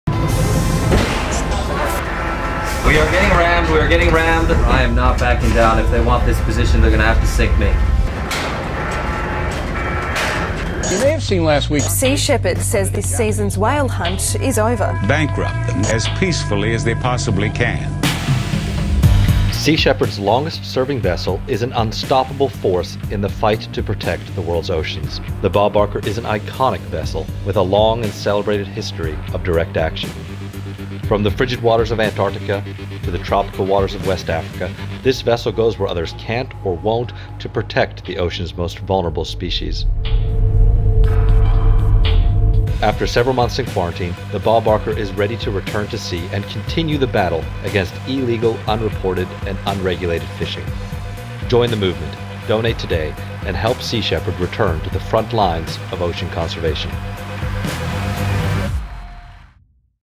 MEET THE FLEET - Bob Barker
Sea Shepherd's longest-serving vessel is an unstoppable force in the fight to protect the world's oceans. After several months in quarantine, the Bob Barker is ready to return to sea and continue the battle against illegal, unreported, and unregulated fishing.